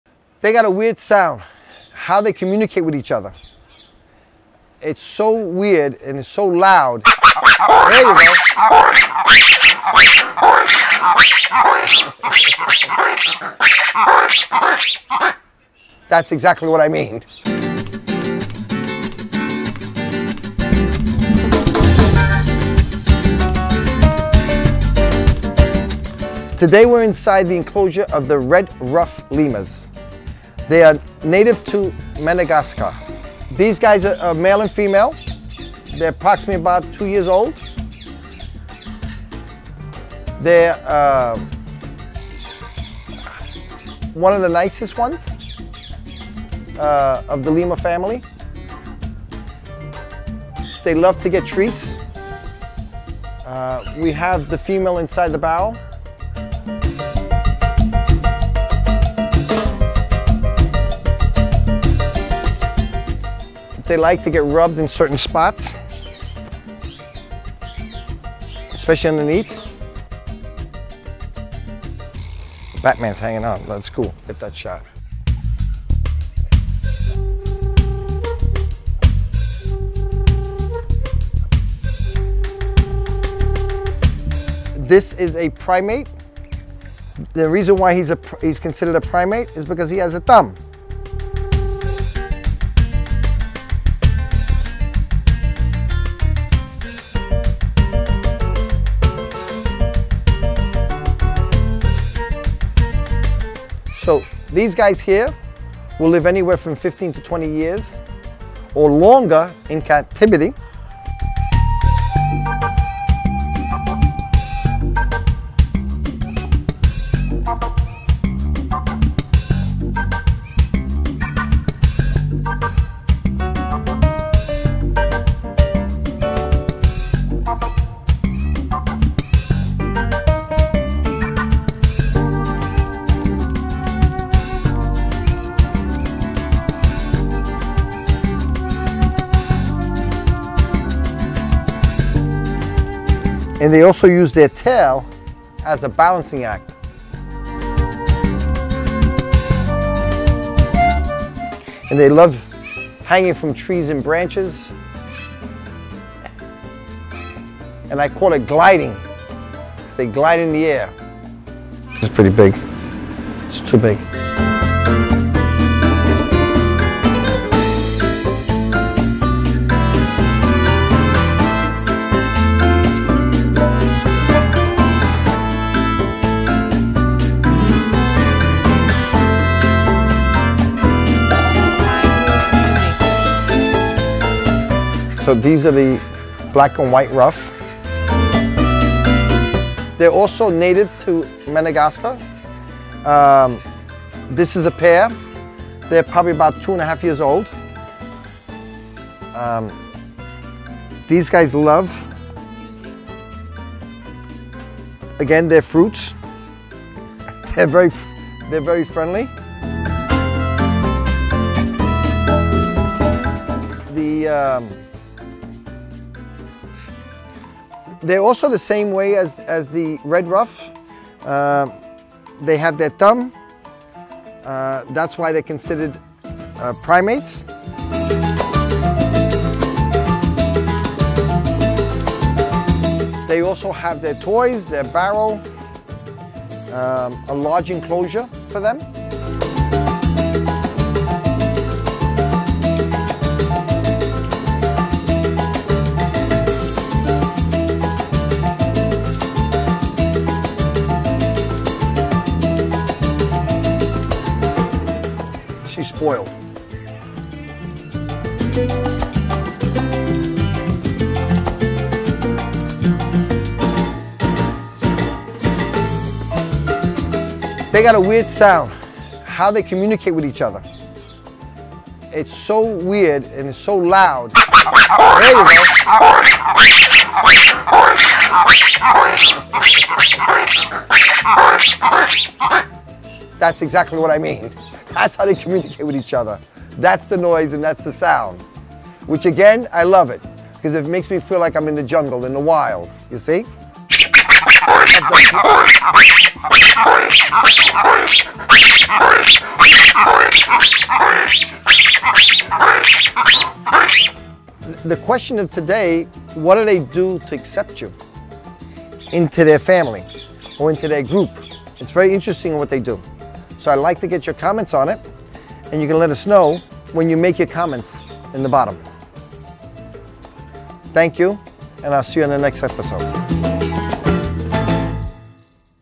Watch my Lemurs go crazy!! // H.L. Tours His Zoo // Red Ruffed and Black-and-White Ruffed Lemurs
What do you think these Lemurs do to accept you into their family???

The Lemurs are one of the largest primates of Madagascar and are known for their loud, raucous calls. The Red-Ruffed Lemur is a very clean animal and spends a lot of time grooming itself. They are known to live 15-20 years in the wild and 25-30 years in captivity.

The Black-and-White Ruffed Lemur is known for their "loud roar/shriek choruses which have several purposes including group movement, spacing among different groups, and alarming other members of the group of predators."

Welcome to H.L.'s Menagerie! An exotic zoo, right in H.L.'s backyard! Take the private tour and you'll see everything from a Black and White Ruff to a Black Palm Cockatoo!

Special thanks to Russel Bay Productions for Camera Assist & Drone Operation